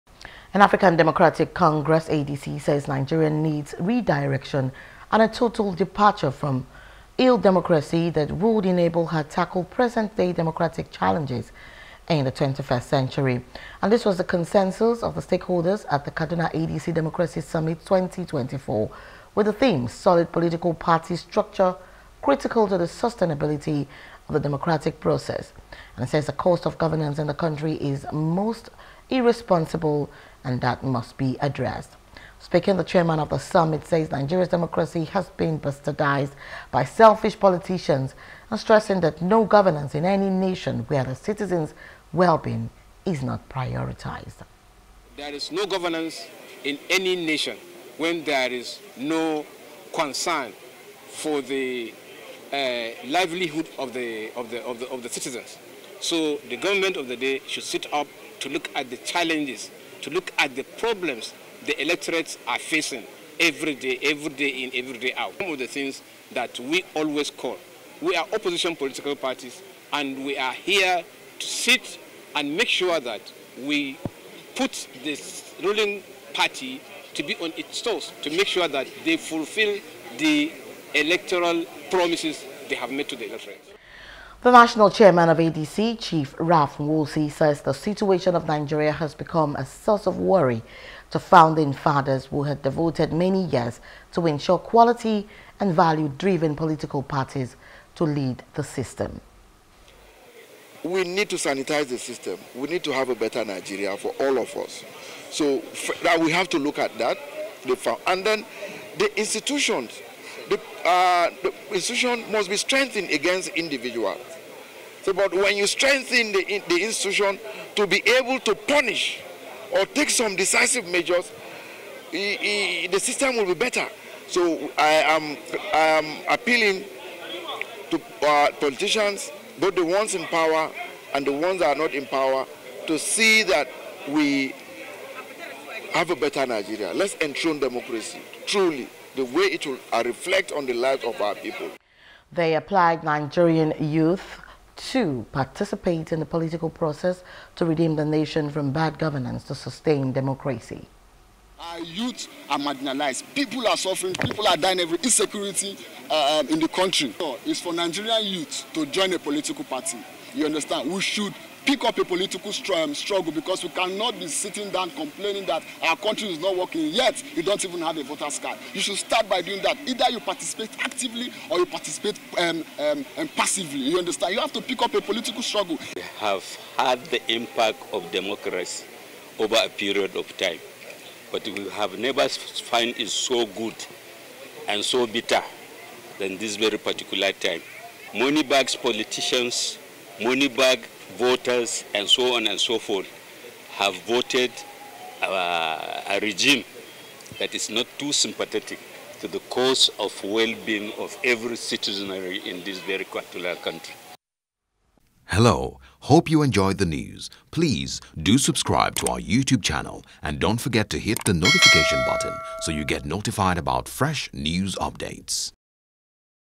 Kaduna Democracy Summit: Nigeria Needs Total Redirection For Democracy To Thrive - A.D.C
African Democratic Congress, (ADC) says Nigeria needs redirection and a total departure from ill democracy that would enable her tackle present day democratic challenges in the 21century.